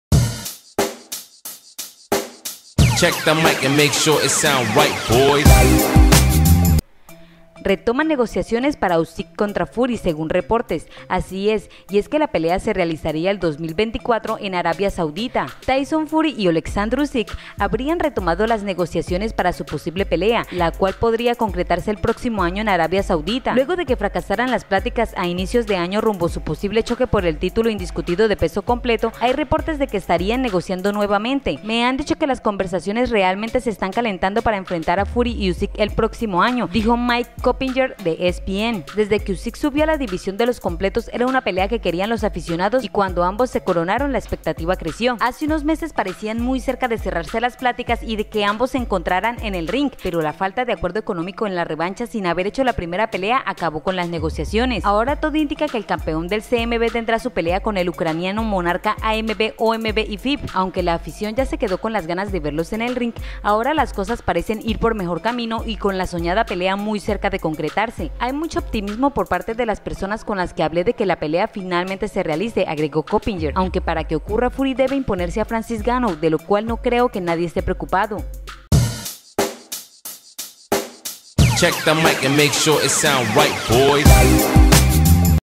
ATENCIÓN! RETOMAN NEGOCIACIONES PARA USYK CONTRA FURY, SEGÚN REPORTES
RETOMAN NEGOCIACIONES PARA USYK CONTRA FURY, SEGÚN REPORTES